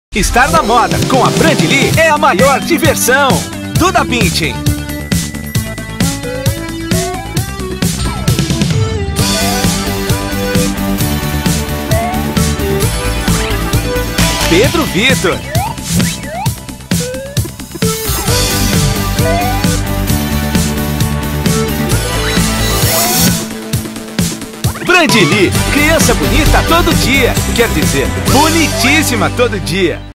Brandili - Comercial Superlativos
Ficha técnica

Agência: e21 - Núcleo Moda e Comportamento
Cliente: Brandili
Produto: Coleção Primavera 2011
Campanha: Superlativos
Direção de Planejamento: Luciano Vignoli
Planejamento: André Dutra, Enéias Brum e Paola Lay
Atendimento: Marina Alvares e Fabrício Soares
Direção de Criação: Dudu Vanoni
Redação: Dudu Vanoni, Lucas Bergallo e Bárbara Bastos
Direção de Arte: Sabrine Sousa, Christine Sassen e Daniel Ramos
Design: Vanessa Meyer e Telma Adams
Produção Gráfica: Maristela Mello e Rebeca Oliveira
Arte-Finalização: Gilberto Bica
Coordenação de Mídia: Diná Lopes
Mídia: Aline Carré
Modelos: Duda Bündchen e Pedro Vítor
Beauty: Antonio de Luccia
Styling: Madeleine Müller
Fotos: Marcus Luconi
Tratamento de Imagem: Flatten Image
Direção Digital: Fernando Sartori
Atendimento Digital: Glauber Gedoz
Gerenciamento do Projeto Web: Glauber Gedoz
Web Design: Adilson Chagas
Redação Web: Guilherme Bueno
Programação: Cléber Alves da Silva e Daniel Wermann
Aprovação: Germano Costa, Mariana Dulac e Fabiano Amaral